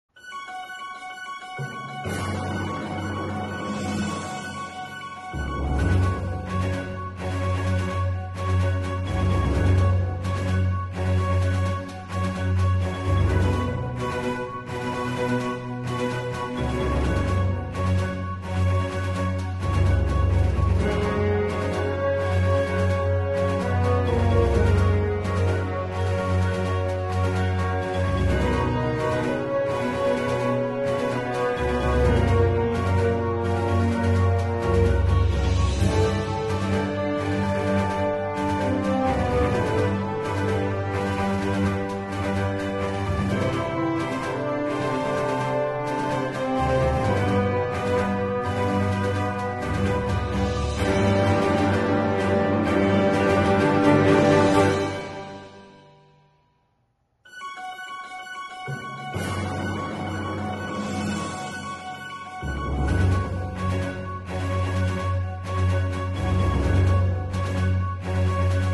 Damascus Airport
Damascus Airport had Became Iran Military Airport